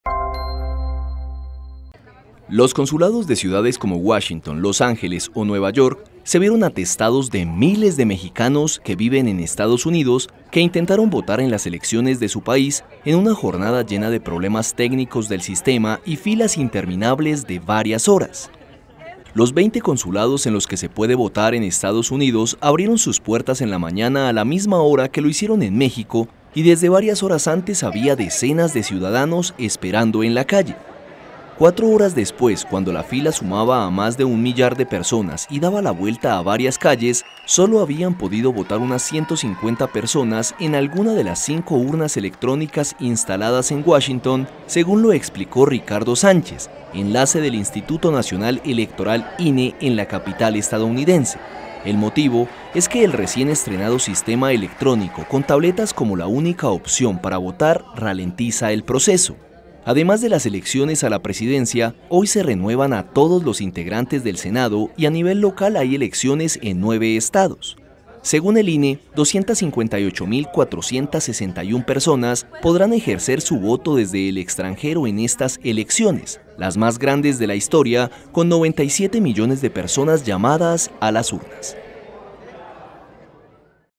Miles de mexicanos residentes en EUA votan entre problemas técnicos y filas interminables
Miles de mexicanos en EE.UU. intentan votar este domingo en las elecciones de su país, en una jornada llena de problemas técnicos del sistema y filas interminables de varias horas en los consulados de ciudades como Washington, Los Ángeles o Nueva York.